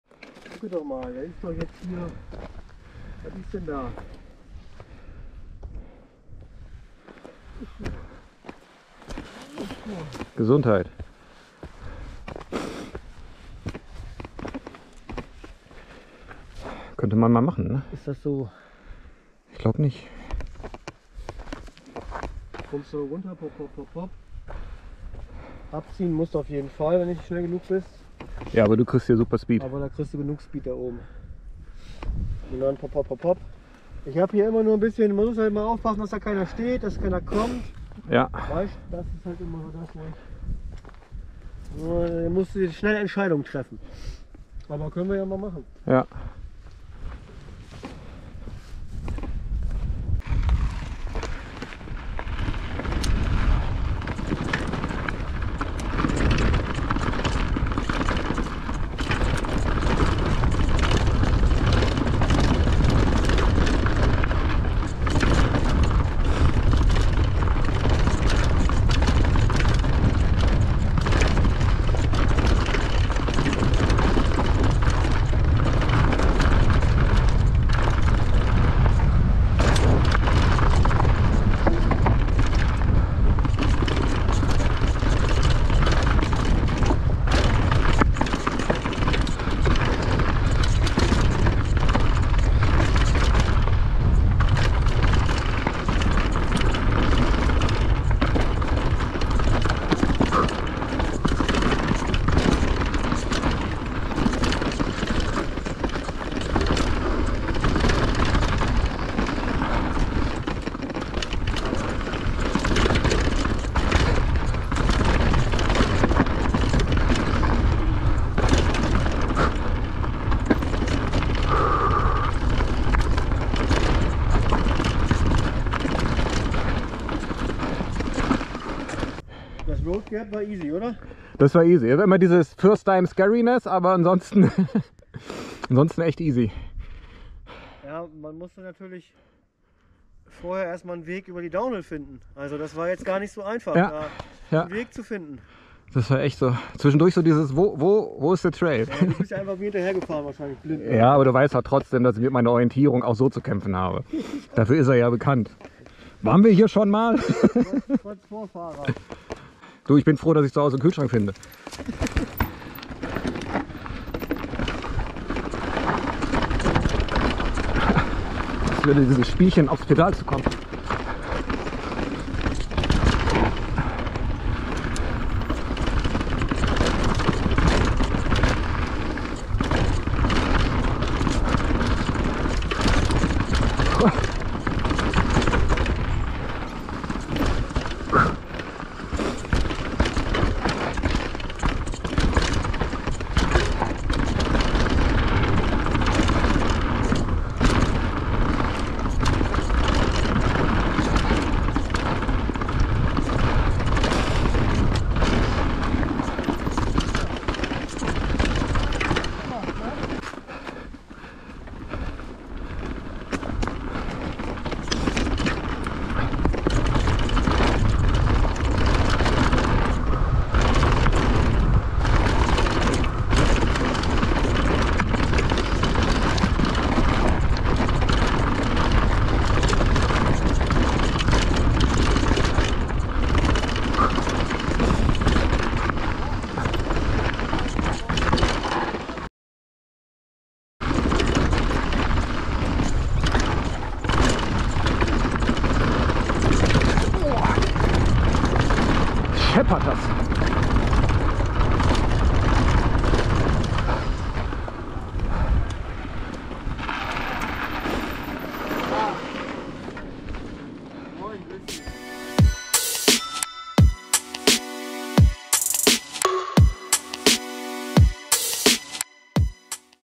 DOWNHILL Schulenberg | DH Bikepark Harz | Propain Rage
DH Abfahrt in Schube top to bottom - mit ein bißchen Trailtalk 😜
Spot: Racepark Schulenberg
Bike: Propain Rage
Cam: GoPro9